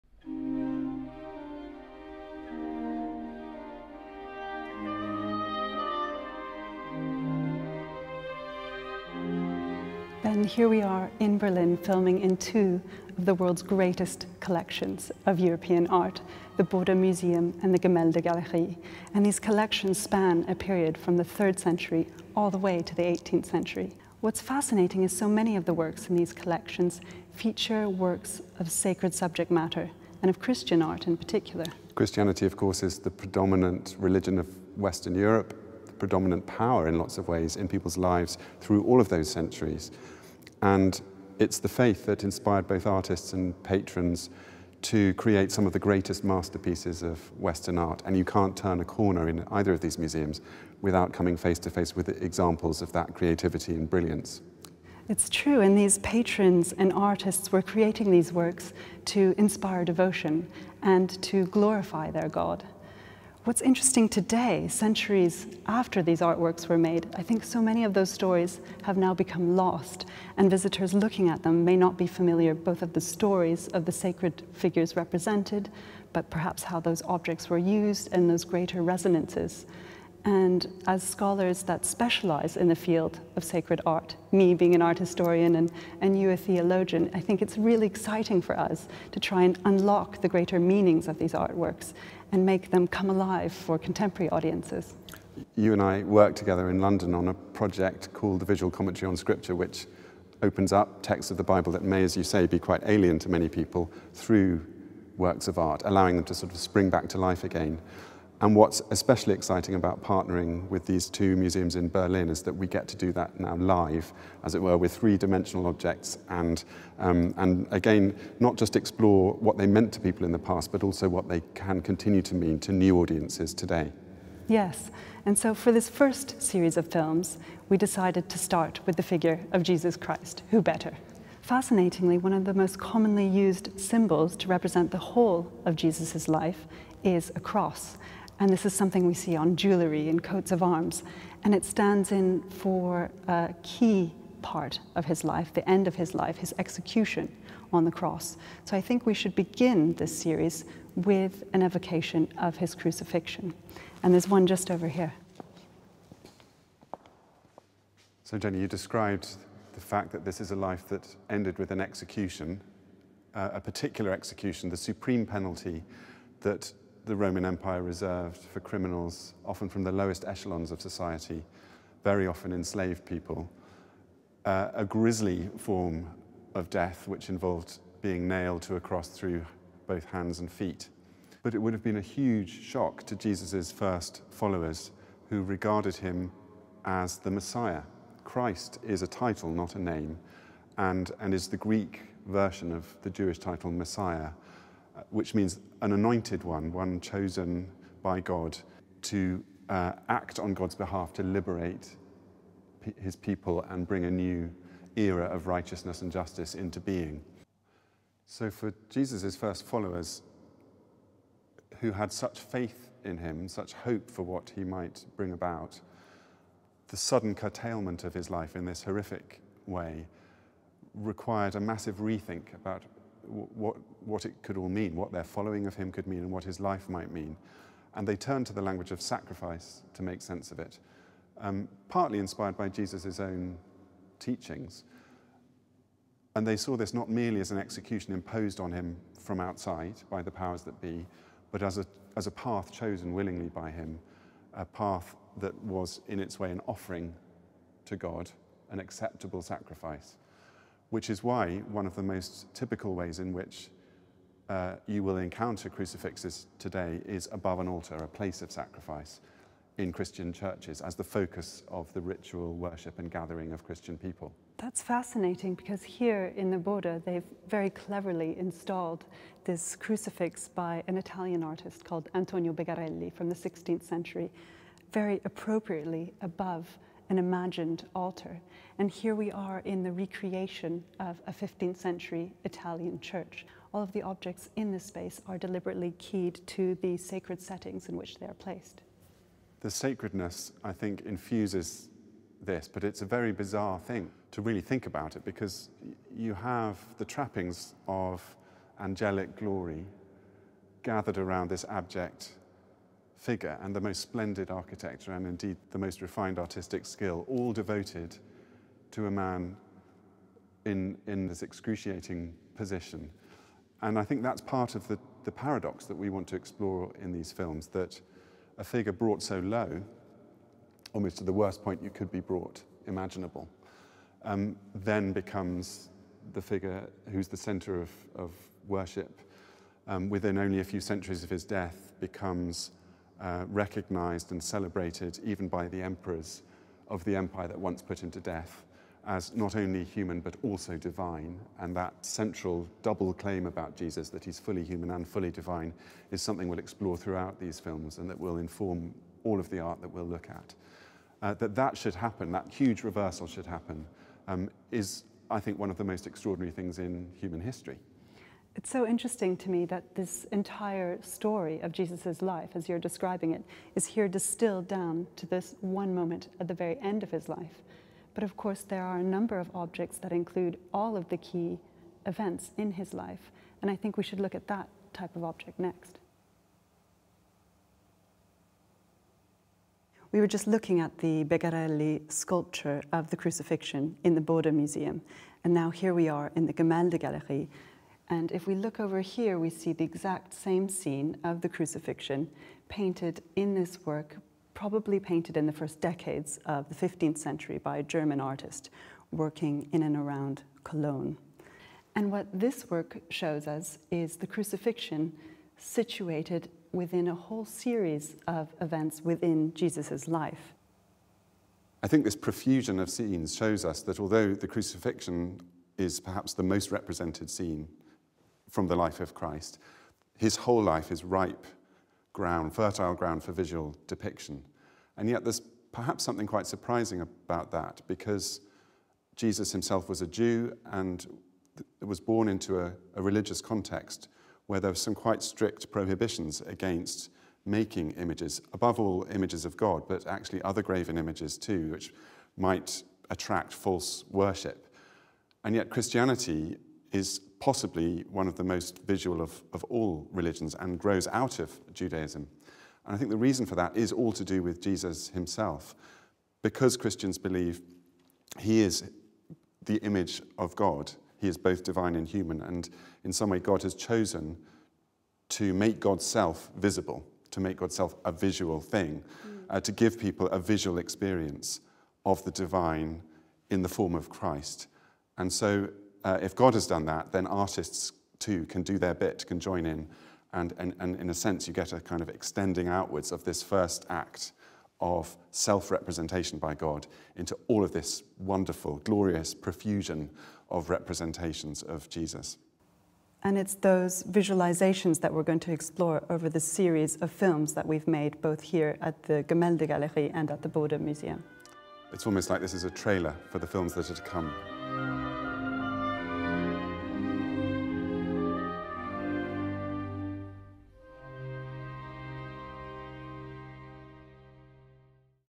Unlocking Christian Art: Jesus Christ. Introduction
The first film in the series focusing on Christianity’s central figure: Jesus Christ.

These films are designed to equip users to understand, read, and enjoy some of Europe’s greatest works of Christian art, as well as the texts that inspired them, and to encourage fresh interpretation and insight in today’s audiences.

This introductory film discusses how Jesus is represented in works of art, from simplified or abstract symbols to complex narratives. It identifies key episodes of Jesus’s life and considers what these events meant and continue to mean for Christians as well as what they might mean for those of different faiths or none who see them in art galleries today.

Presented by VCS Director Professor Ben Quash and Deputy Director Dr Jennifer Sliwka.

Artworks: The Bode Basilica; Antonio Begarelli, Crucifixion with Angels, 16th century, both in the Bode-Museum and Cologne, Life of Christ, c.1410/20 in the Gemäldegalerie, Berlin.